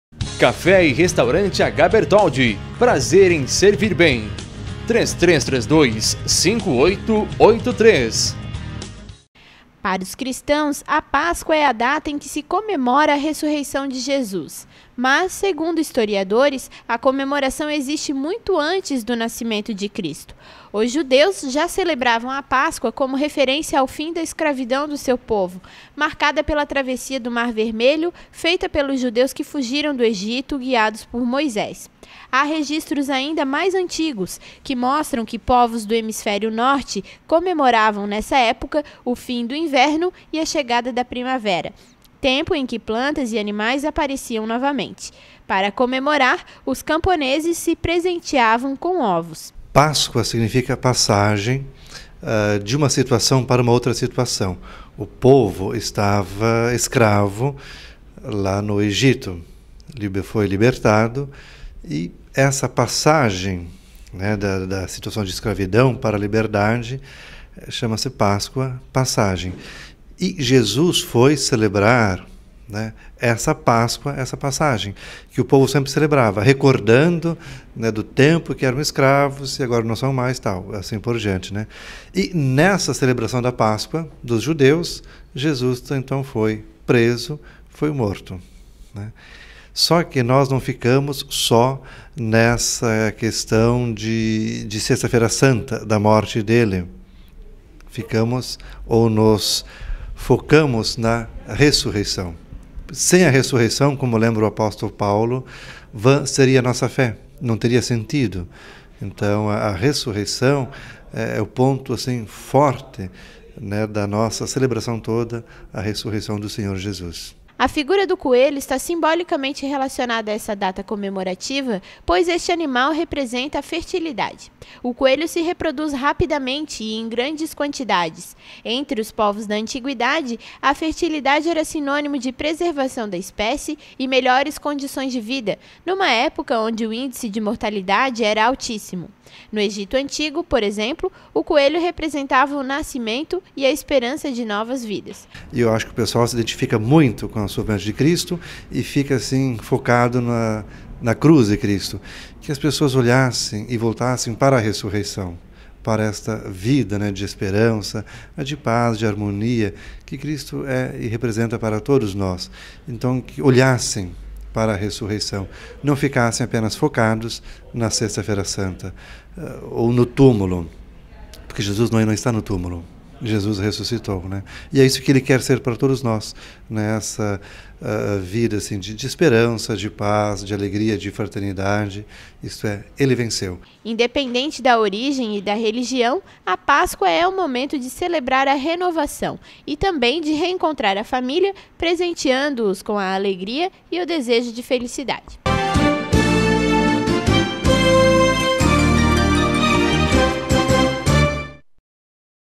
Porque a Páscoa tem ovos e coelhinhos
Para os Cristãos, a Páscoa é a data em que se comemora a Ressurreição de Jesus. Mas, segundo Historiadores, a comemoração existe muito antes do Nascimento de Cristo